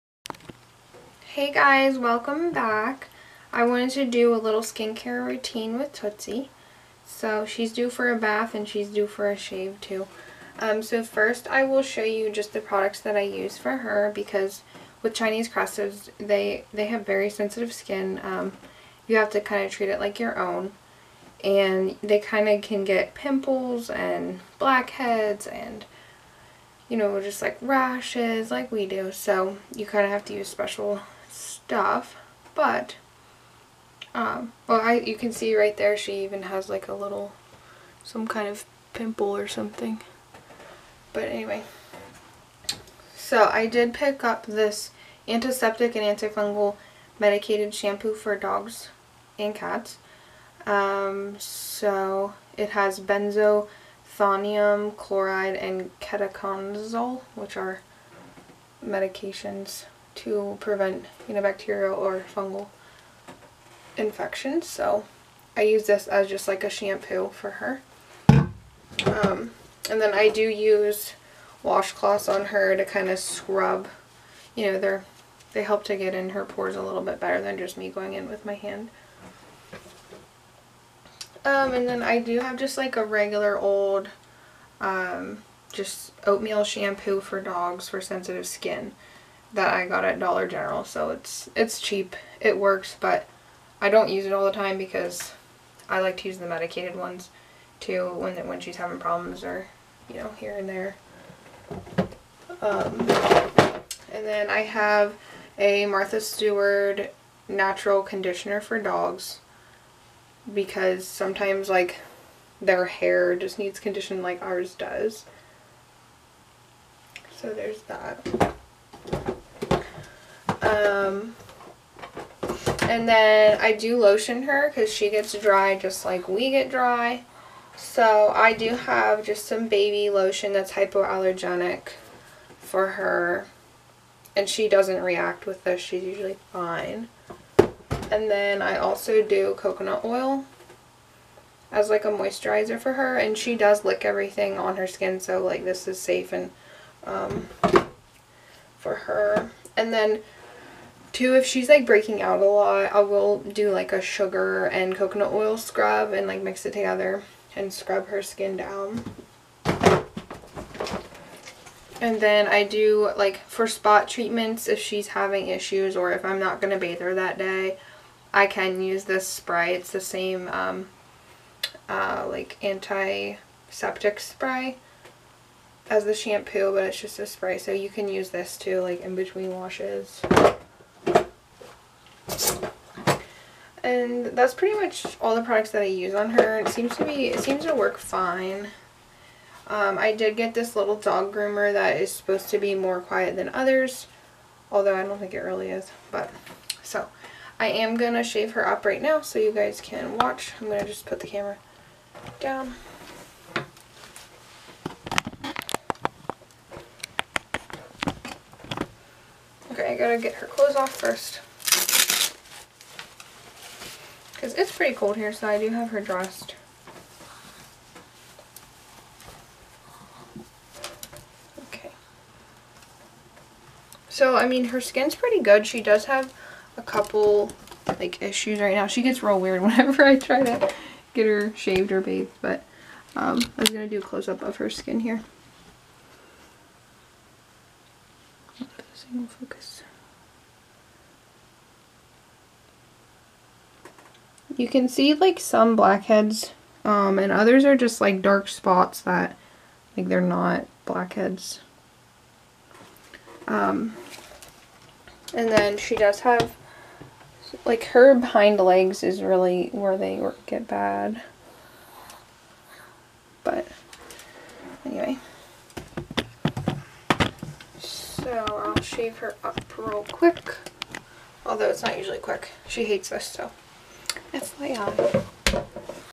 Chinese Crested skin care routine
This is just some of the products I use on my pup with her sensitive skin. Comment with any videos you would like to see with Tootsie the chinese crested!